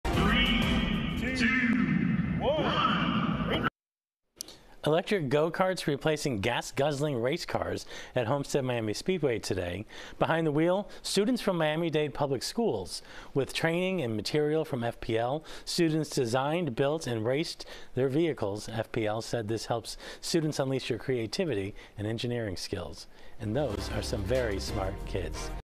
Miami-Dade students go electric go-kart racing at Electrathon in Homestead
Electric go-karts replaced gas-guzzling race cars at the Homestead Miami Speedway on Saturday with Miami-Dade Public School students behind the wheel during the Electrathon.